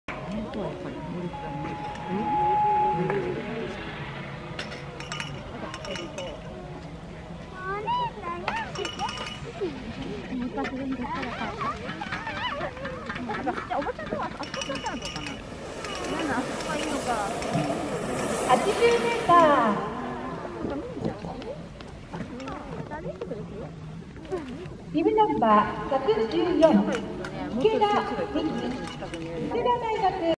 三上琳加 選手 第101回全日本ラージ １本目 80メーター
余市紅志高１年のリンカ選手。１本目は、女子組22位。
2022/10/23(日) 白馬ジャンプ競技場